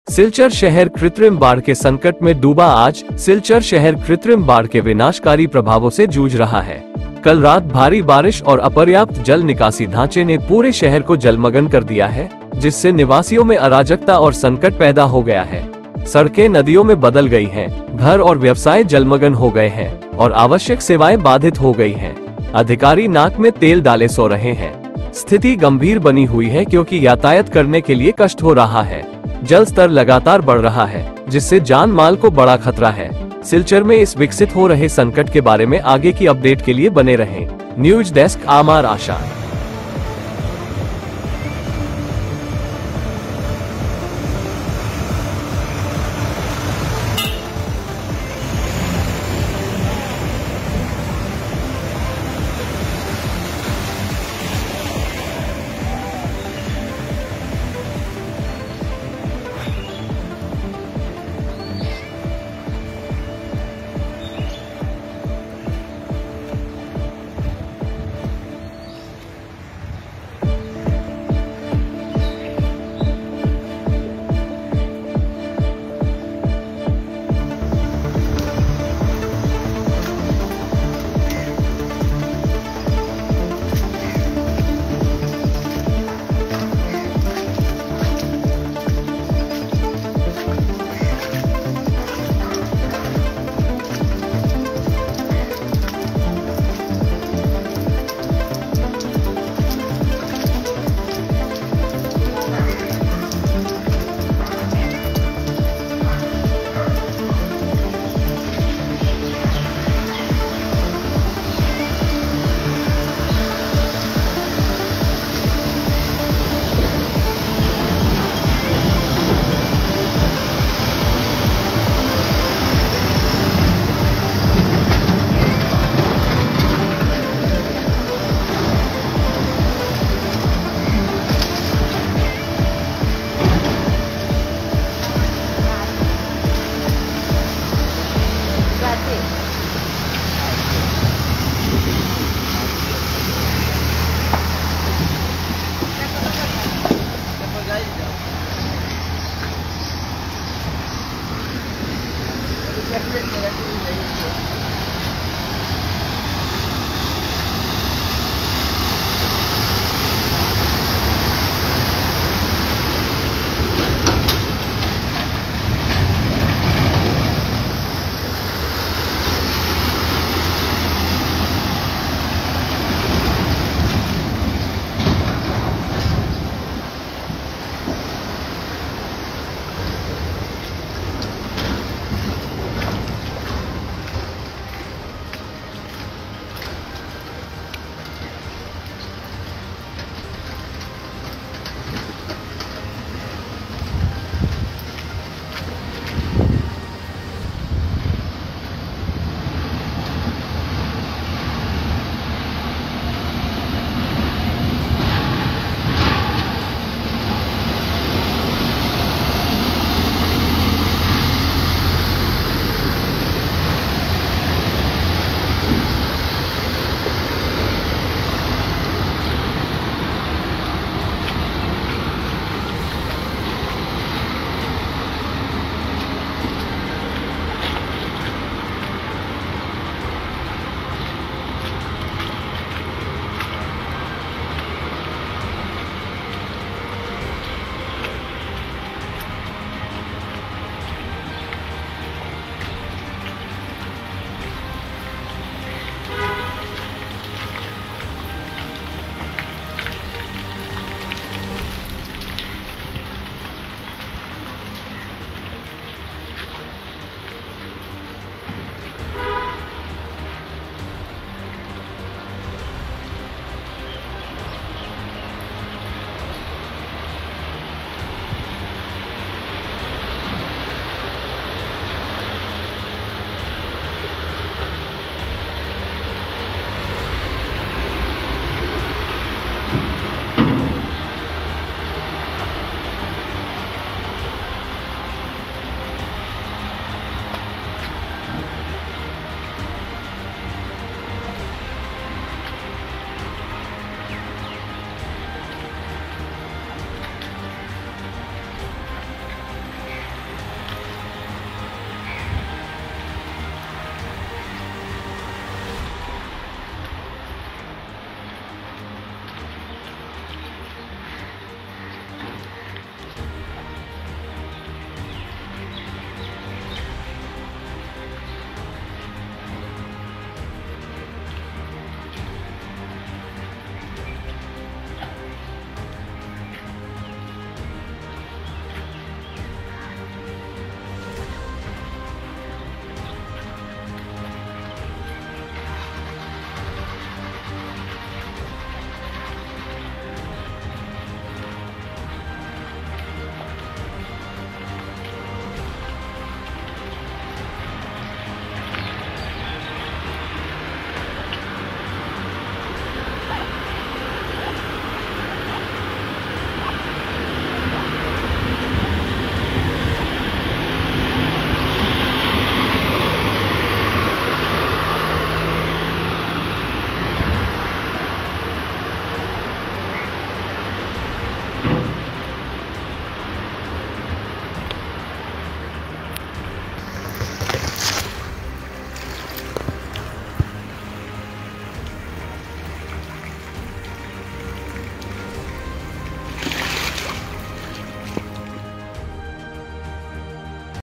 सिलचर शहर कृत्रिम बाढ़ के संकट में डूबाआज सिलचर शहर कृत्रिम बाढ़ के विनाशकारी प्रभावों से जूझ रहा
ब्रेकिंग न्यूज़: सिलचर शहर कृत्रिम बाढ़ के संकट में डूबा
आज, सिलचर शहर कृत्रिम बाढ़ के विनाशकारी प्रभावों से जूझ रहा है।